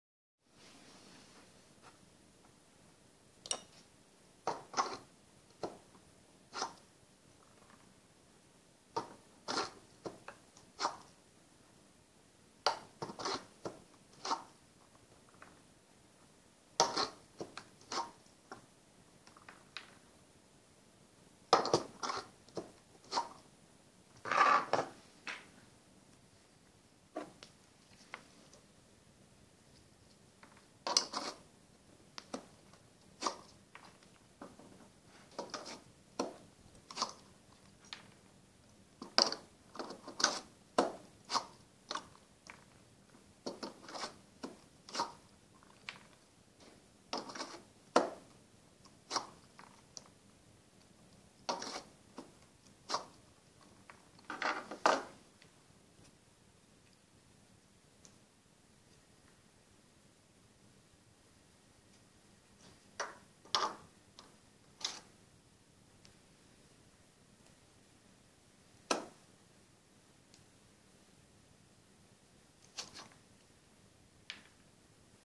cutting rubber feet from o-ring cord
I made a small cutting tool to make lots of rubber plug feet... lost count of them some time ago ;-)